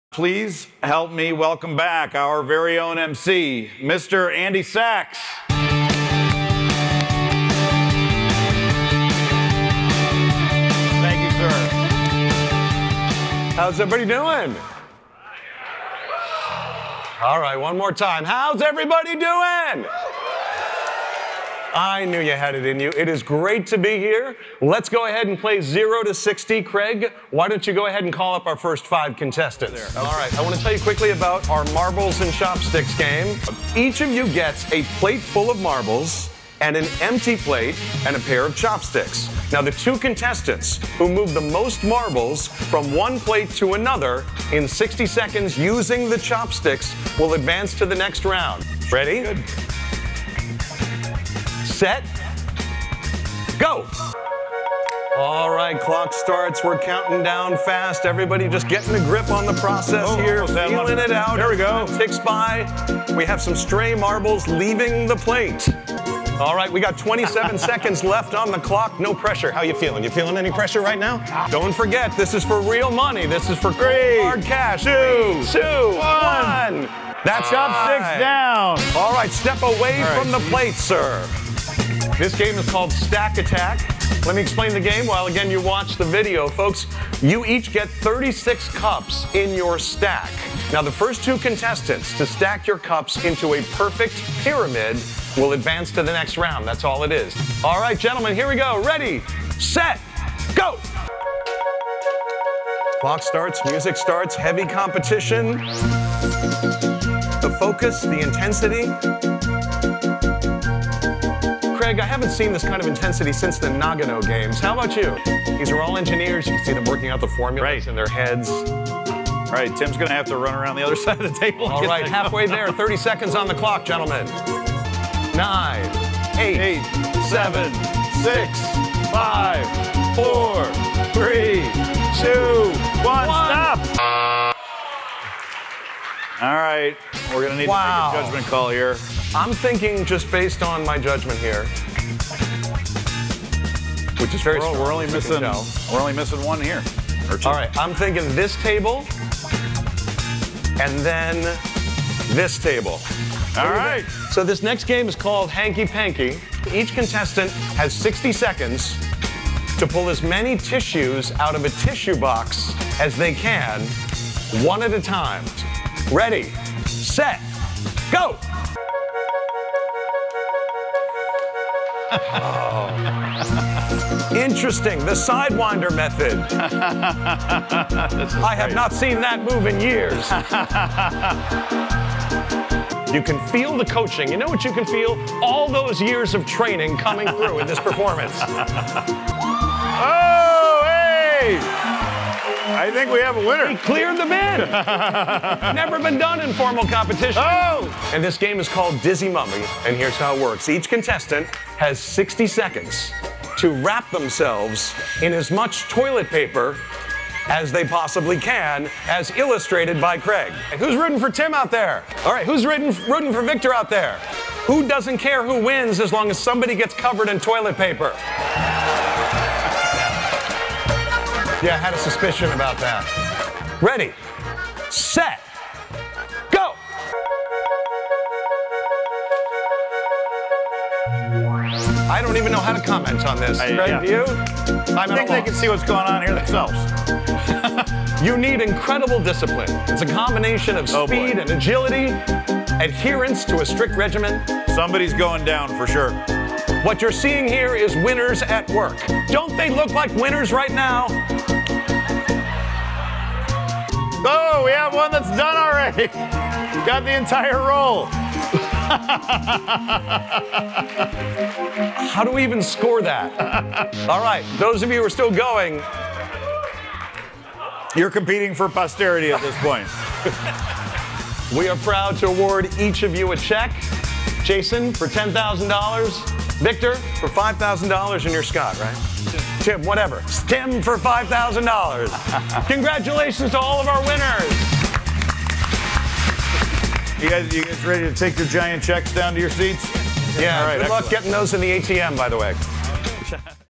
★ Emcee Games: quick, silly audience-participation games (feat. MC Andy Saks)
0:30 Marbles & Chopsticks
1:22 Stack Attack
2:34 Hanky Panky
3:18 Dizzy Mummy

This video shows highlights of professional emcee Andy Saks facilitating four short, fun audience-participation games in front of 2,800 people at a high-profile corporate conference at the Anaheim Convention Center in Anaheim, California, in 2018.